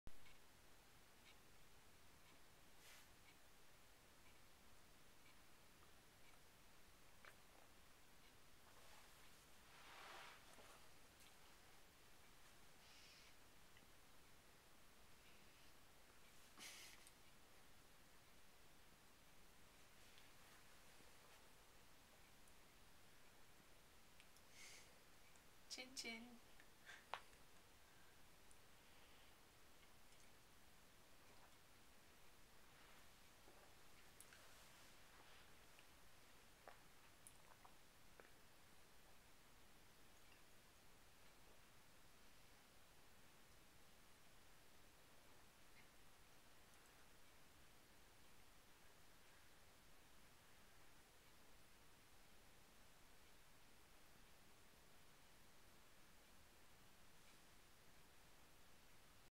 Inglenook
Home video at night in front of the inglenook fireplace, with wine and candles.